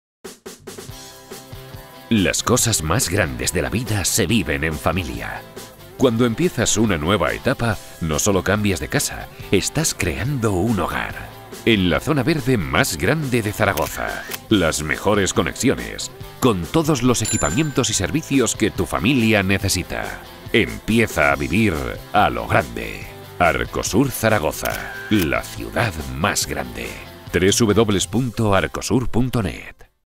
ARCOSUR SPOT TV 30"
Spot para la campaña de la promoción inmobiliaria  Arcosur, todavía sin terminar, se espera que sea para el 2015 pero de la que se entregan las primeras  llaves en septiembre del 2012.

Realización y cámara.

Texto  y producción a cargo de Essentia Comunicación.